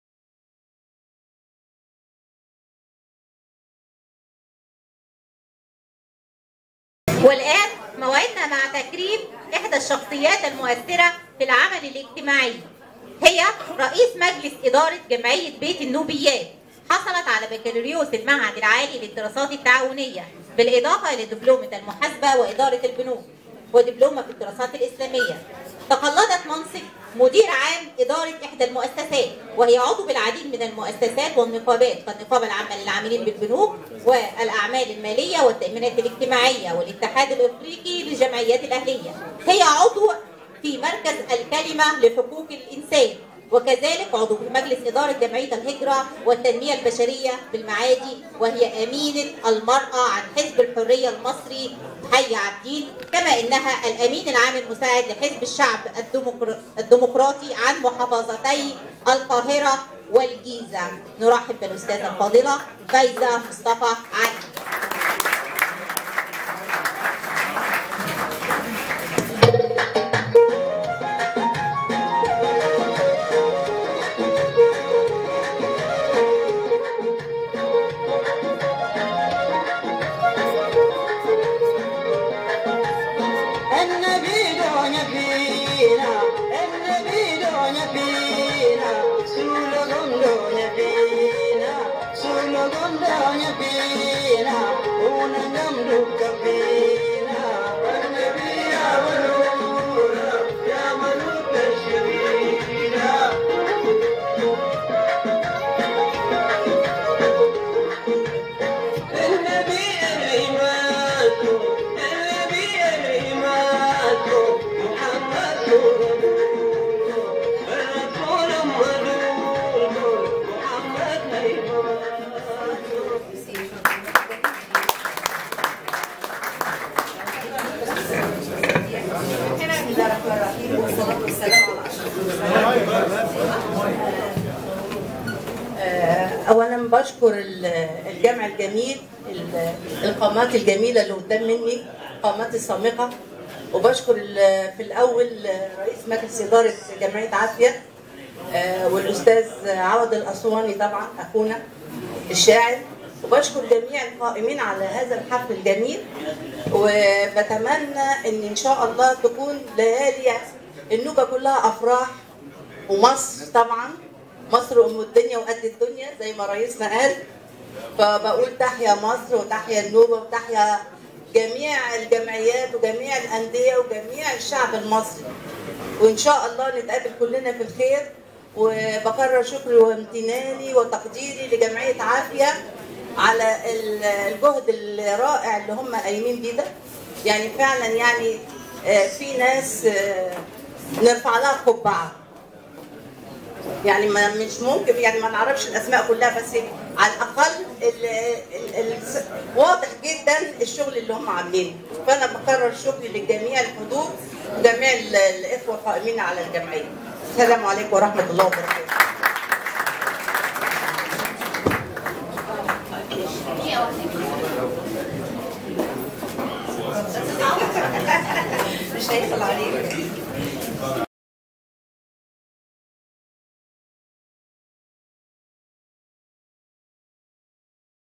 جائزة أحمد ادريس/تكريم/أ/فايزة مصطفى/السبت14/10/2023/نادى العاملين بالاتصالات/كورنيش النيل /العجوزة
حفل سنوي / جائزة أحمد ادريس / السنة الأولى
صاحب شفرة حرب أكتوبر 1973م باللغة النوبية
جائزة التميز والإبداع والإبتكار
جمعية عافية النوبية بالقاهرة / عائلة أحمد إدريس
إدارة المنصة الإعلامية والشاعرة  / مرفت شوقي
  والإعلامية الكاتبة / منال غانم مكي
تكريم الأستاذة /فايزة مصطفى علي
المكان : نادى العاملين بالاتصالات/كورنيش النيل/حي العجوزة
الزمان : السبت 2023/10/14
تصوير وتوثيق شاعر الصورة كابتن / دهب المجرابي